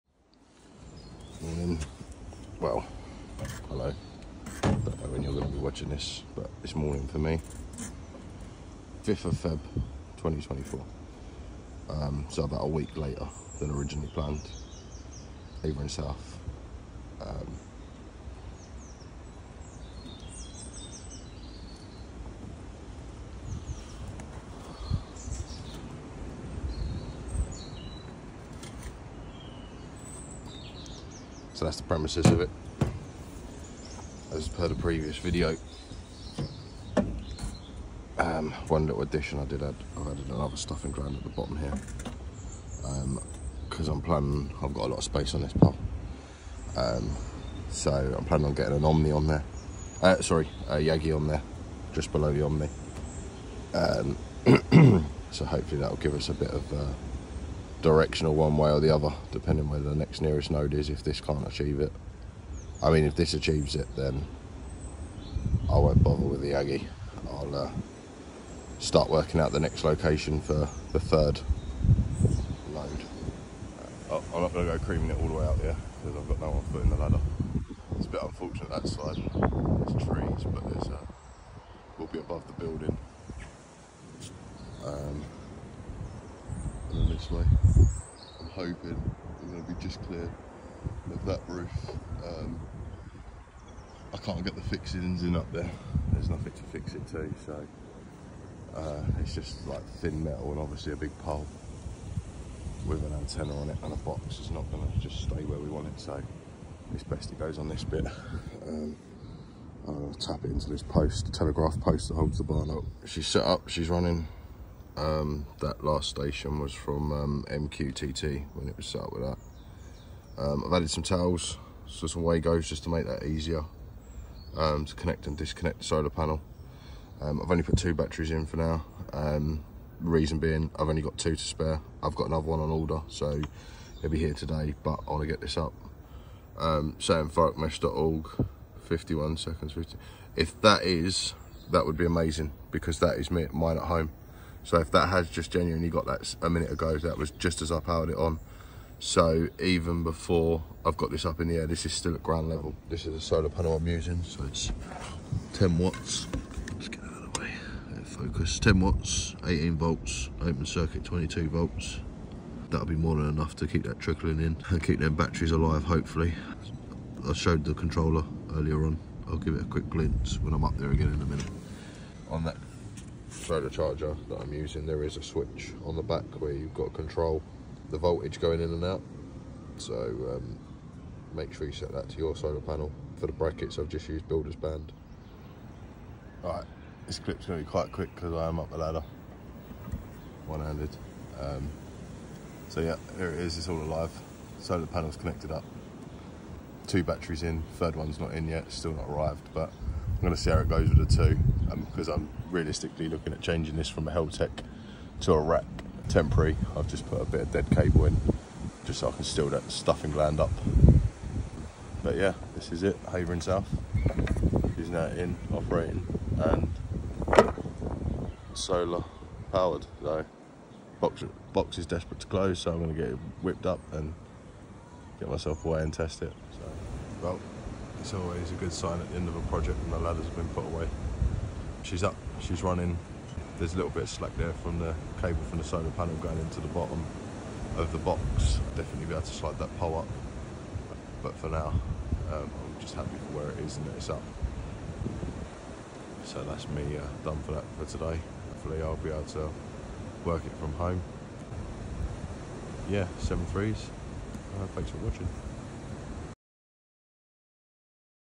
MESHTASTIC SOLAR NODE PART 2 INSTALLATION
Part 2 of my Solar Node build for Meshtatsic. Built using a Heltec V3 and 18V solar panel, Havering_South sits on the Havering & Thurrock border to bridge the two boroughs together.